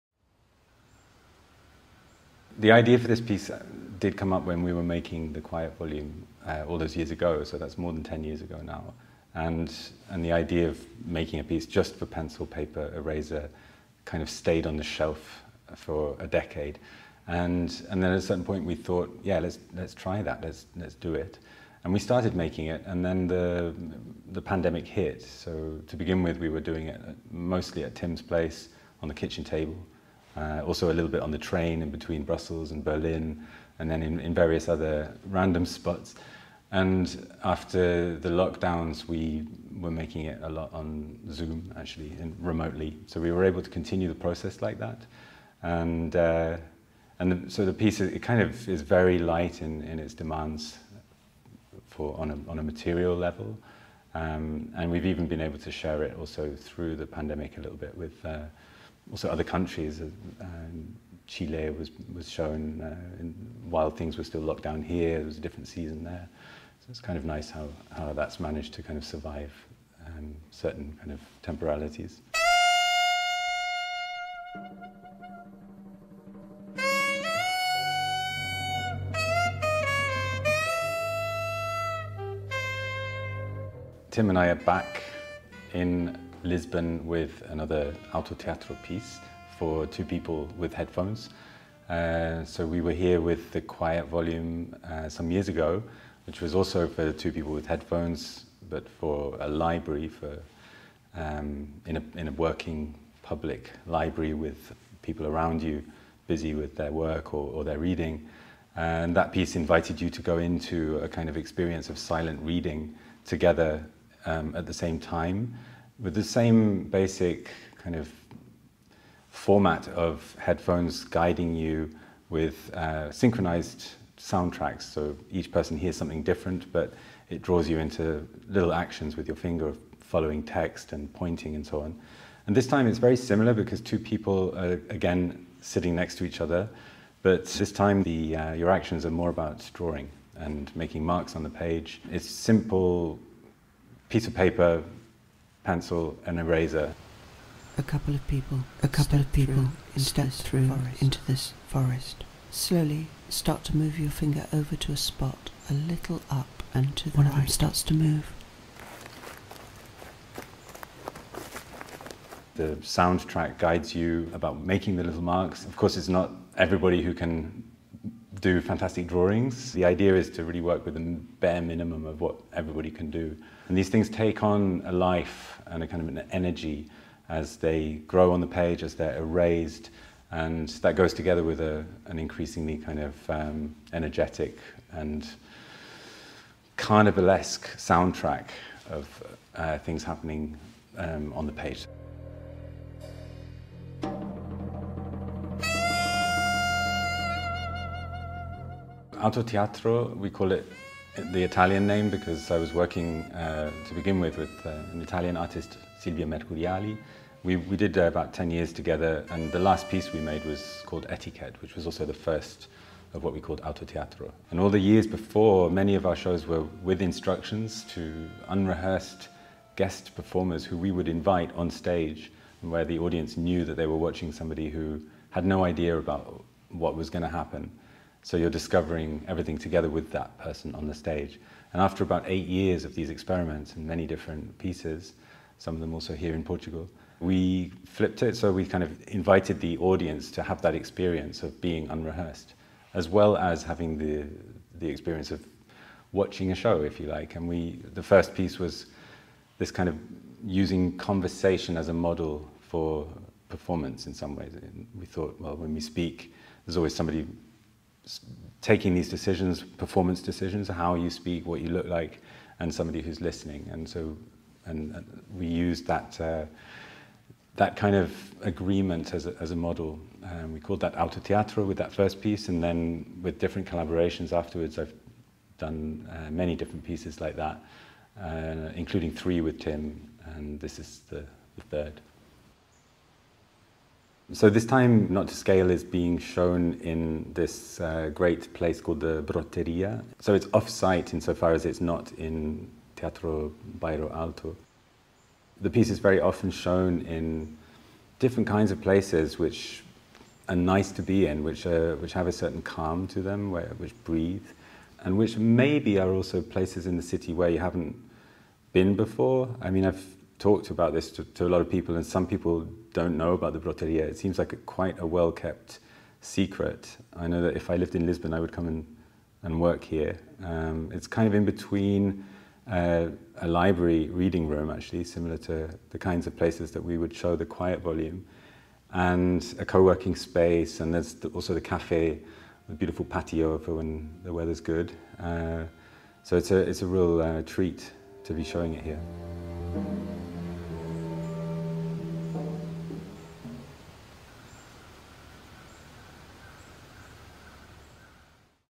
ΚΘΒΕ 2022: «Not to Scale», Ant Hampton talking about autoteatro
4ο Διεθνές Φεστιβάλ Δάσους

«Not to Scale»
των Tim Etchells και Ant Hampton

Με εργαλεία λίγα λευκά χαρτιά, ένα μολύβι και μια γόμα, το «Not to Scale» είναι μια παράσταση Αutoteatro που καλεί δύο συμμετέχοντες να πειραματιστούν και να εμπλακούν σε μια κοινή εμπειρία, σχεδιάζοντας, σβήνοντας και ακούγοντας.